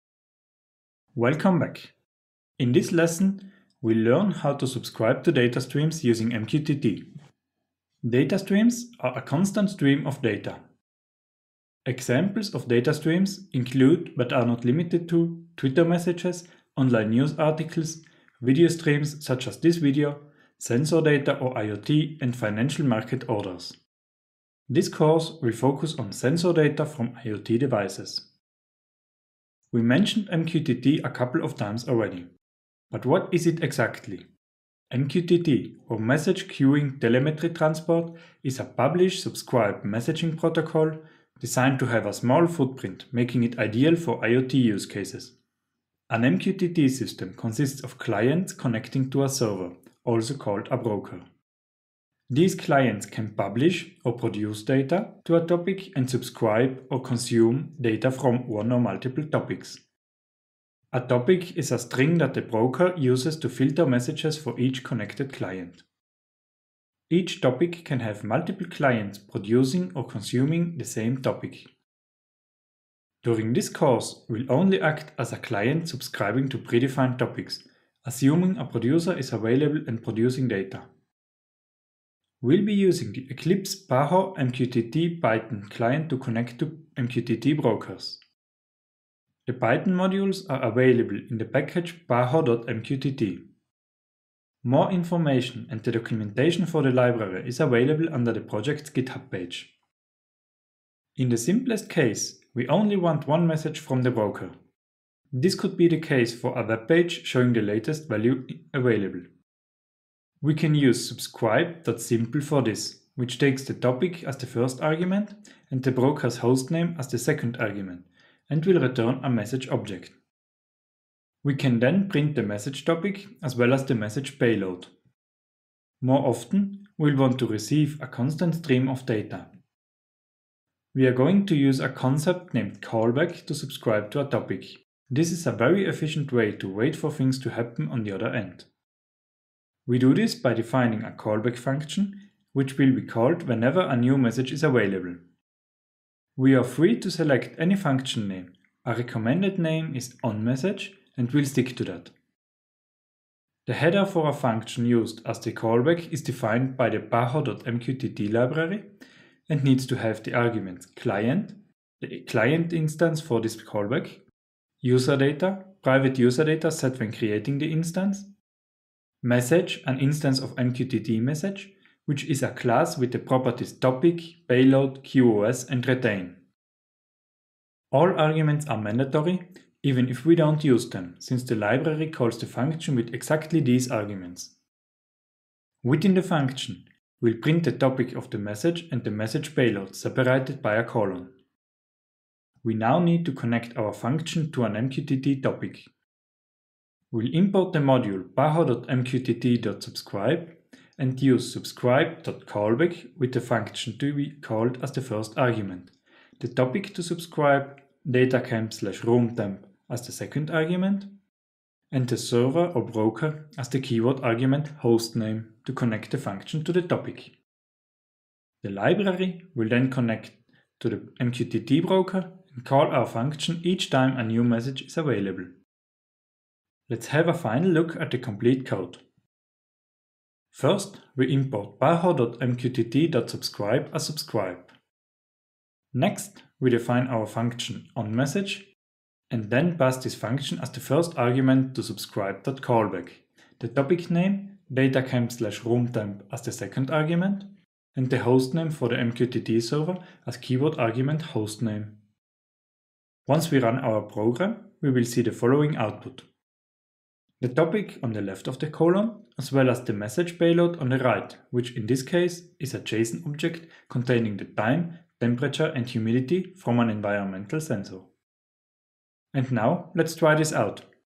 Python Tutorial: Introduction to Data streams
Want to learn more? at your own pace. More than a video, you'll learn hands-on coding & quickly apply skills to your daily work.

Welcome back. Data streams are constant streams of data. Examples of data streams include, but are not limited to Twitter messages, online News Articles, Video streams such as this video, sensor data or IoT, and financial Market orders.

This course will focus on sensor data from IoT devices.

We mentioned MQTT a couple times already. But what is it exactly?

An MQTT system consists of clients connecting to a server, also called a Broker.

A topic is a string that the Broker uses to filter messages for each connected client. Each topic can have multiple clients producing or consuming the same topic.

We'll be using the Eclipse Paho MQTT Python client to connect to MQTT Brokers.

The Python modules are available in the package paho.mqtt.

More information and the documentation for the library is available under the projects Github page.

In the simplest case, we only want one message from the Broker.
This could be the case for a webpage showing the latest value available.

We can then print the message topic, as well as the message payload.

More often, we'll want to receive a constant stream of data.

This is a very efficient way to wait for things to happen on the other end.

We do this by defining a callback function, which will be called whenever a new message is available.

We are free to select any function name. A recommended name is "on_message", and we'll stick to that.

The header for a function used as the callback is defined by the paho.mqtt library and needs to have the arguments client, the client instance for this callback userdata, private user data set when creating the instance message, an instance of MQTTMessage, which is a class with the properties topic, payload, qos, and retain. All arguments are mandatory even if we don't use them, since the library calls the function with exactly these arguments.

Within the function, we'll print the topic of the message and the message payload, separated by a colon.

We now need to connect our function to an MQTT topic.

We'll import the module "paho.mqtt.subscribe" and use subscribe.callback, with  the function to be called as the first argument,  the topic to subscribe ("datacamp/roomtemp") as the 2nd argument and the server or broker as keyword argument `hostname` to connect the function to the topic.

The library will then connect to the MQTT broker, and call our function each time a new message is available.

Let's have a final look at the complete code.

Next, we define our function on_message and then pass this function as the first argument to subscribe.callback, the topic name, "datacamp/roomtemp", as the second argument, and the hostname for the mqtt server as keyword argument "hostname".

Once we run our program, we'll see the following output.

The topic on the left of the colon, as well as the message payload on the right, which, in this case, is a json object containing the time, temperature and humidity from an environmental sensor.

And now, let's try this out. Enjoy!